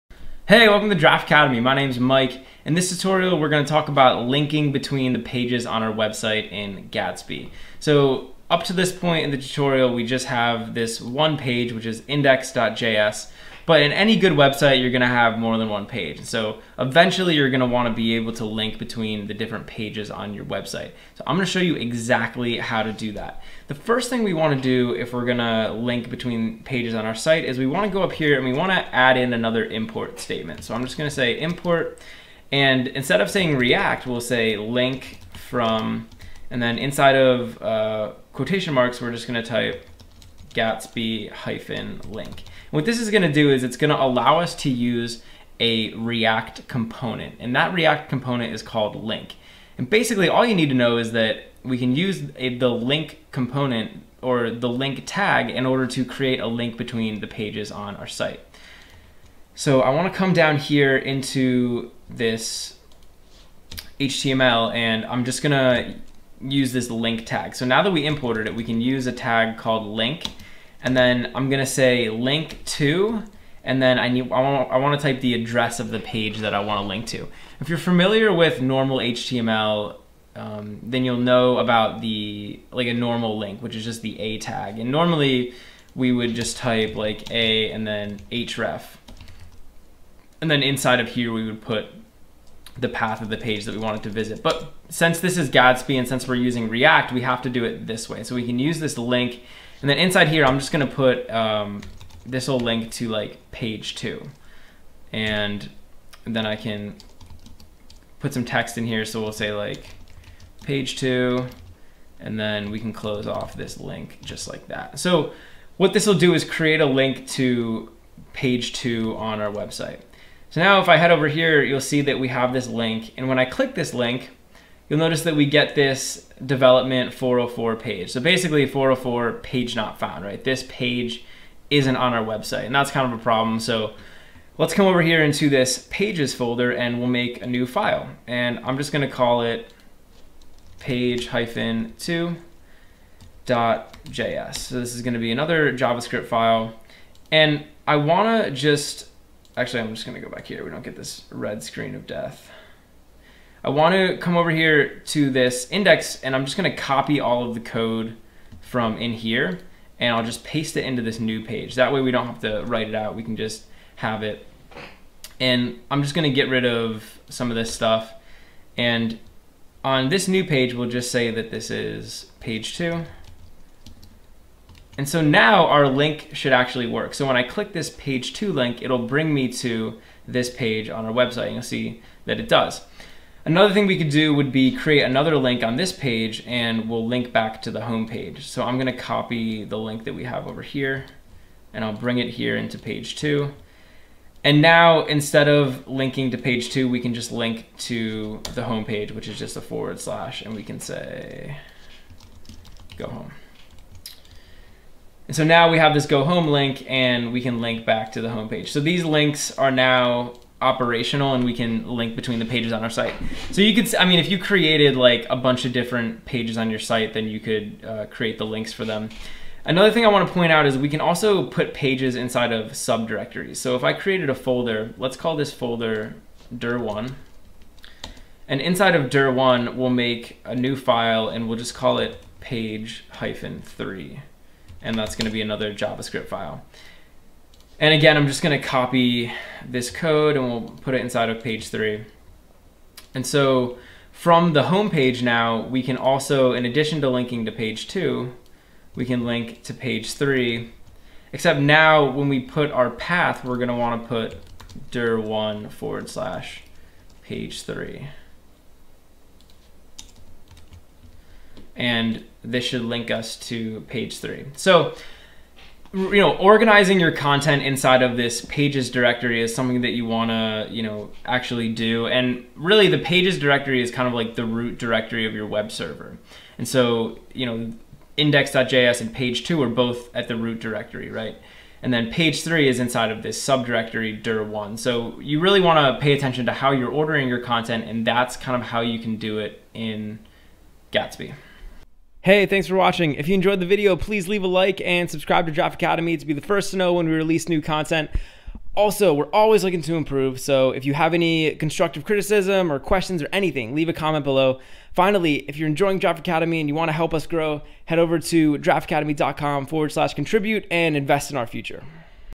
Linking Pages | Gatsby Static Site Generator | Tutorial 5
In this tutorial, Mike will show you how to create links between the different pages on your website